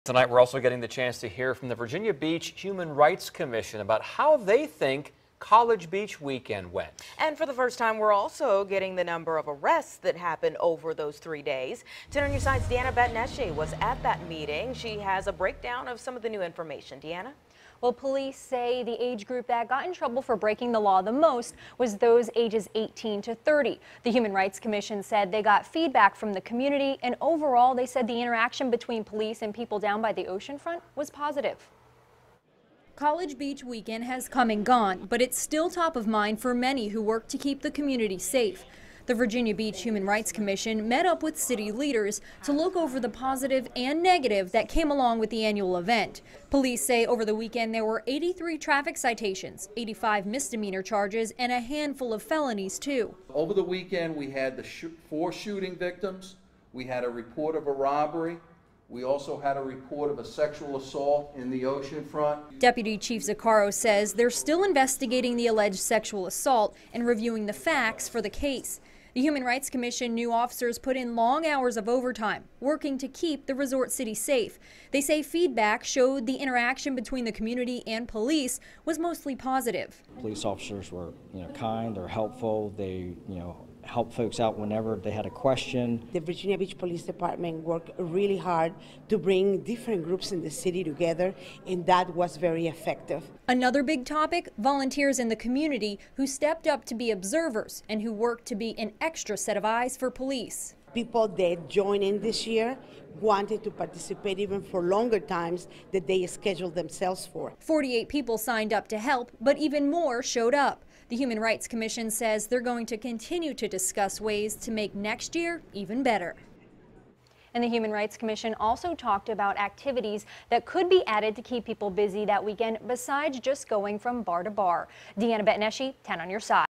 Virginia Beach Human Rights Commission reviews College Beach Weekend
Deanna Bettineschi reports.